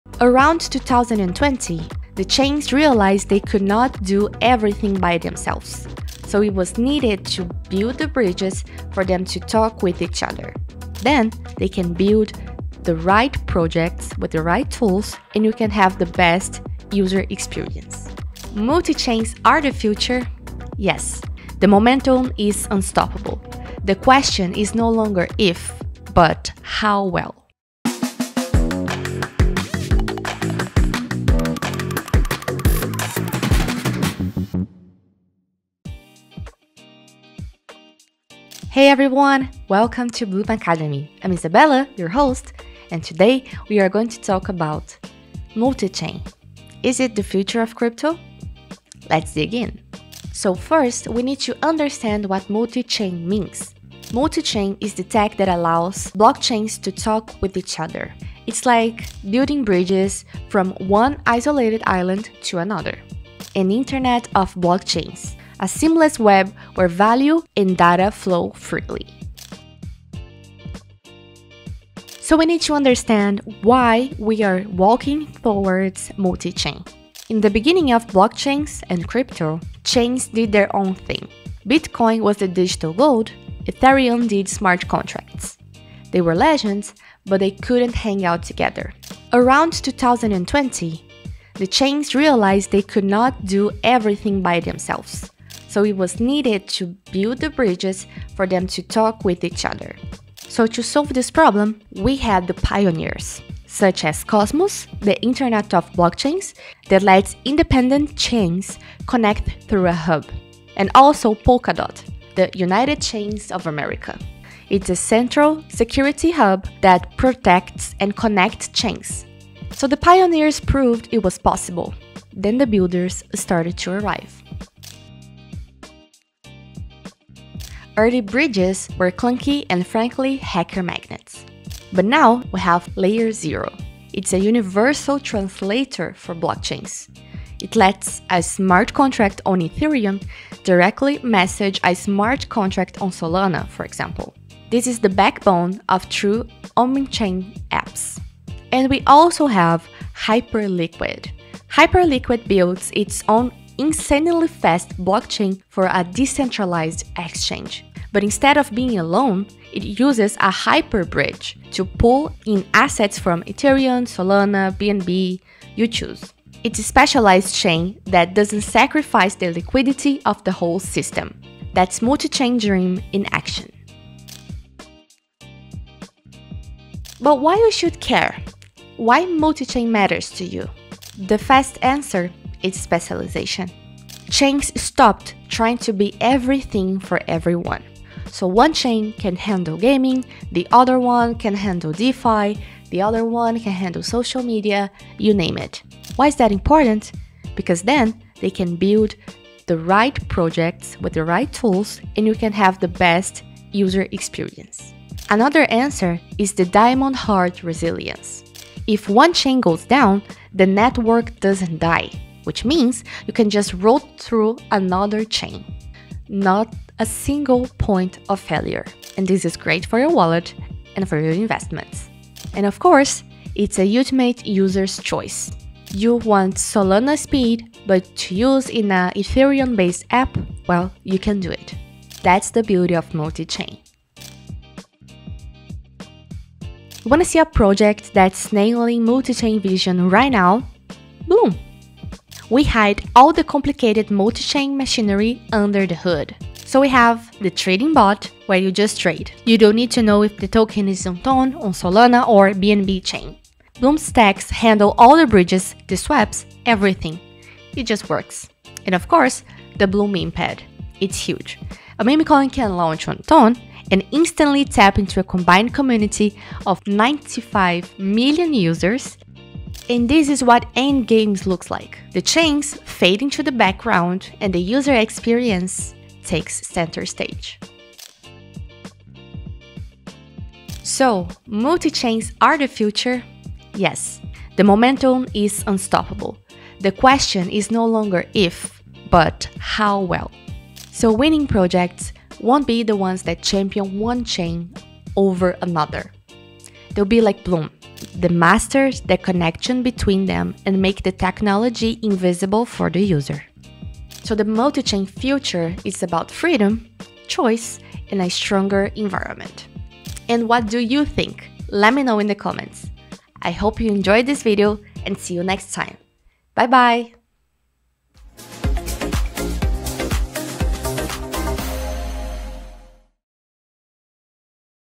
Why Crypto’s Future Won’t Be on Just One Blockchain
In this episode of Blum Academy, we explore how the crypto world is evolving from isolated blockchains to an interconnected multichain ecosystem. Learn what “multichain” really means, how projects like Cosmos, Polkadot, and LayerZero are building it, and why this shift could redefine trading, DeFi, and token launches forever.